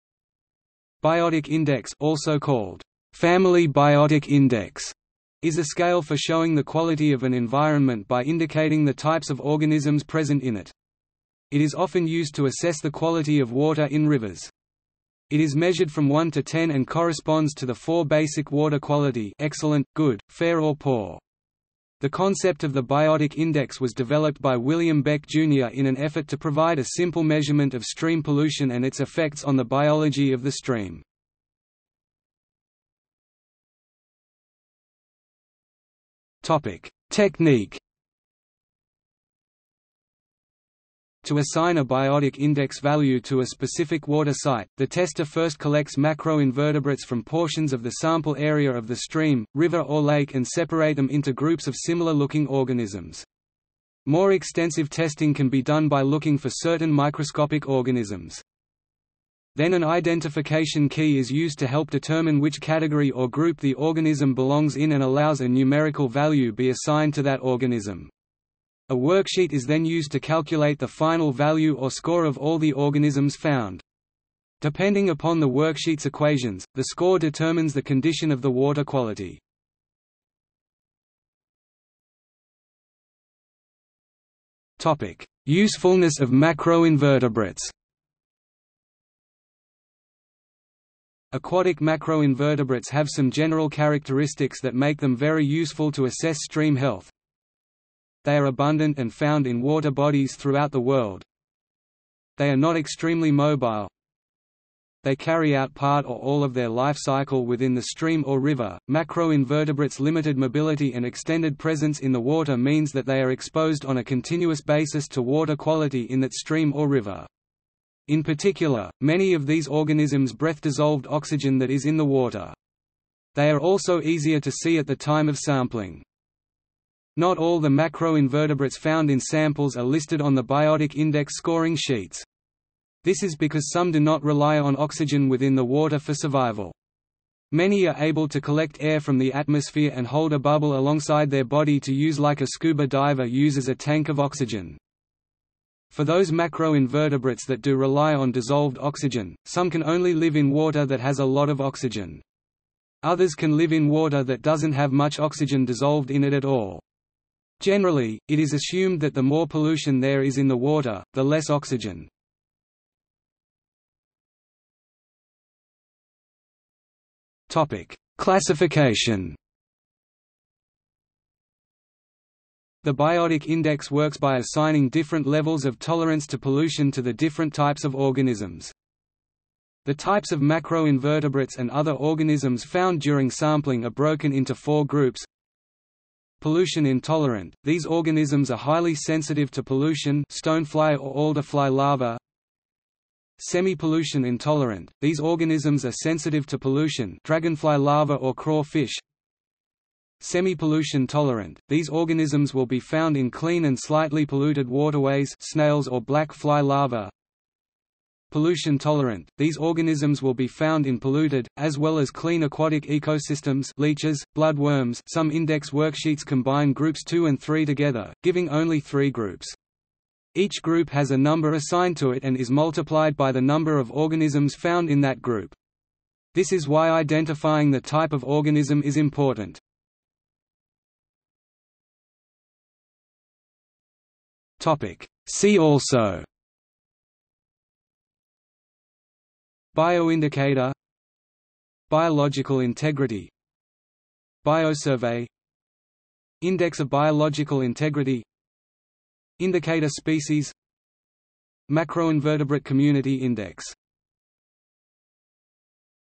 Biotic index | Wikipedia audio article
00:00:32 1 Technique
00:01:21 2 Usefulness of macro invertebrates
00:02:44 3 Classification
00:03:51 4 See also

- Socrates

SUMMARY
Biotic Index (also called "Family Biotic Index") is a scale for showing the quality of an environment by indicating the types of organisms present in it. It is often used to assess the quality of water in rivers. It is measured from 1 to 10 and corresponds to the four basic water quality (Excellent, Good, Fair or Poor). The concept of the Biotic Index was developed by William Beck Jr in an effort to provide a simple measurement of stream pollution and its effects on the biology of the stream.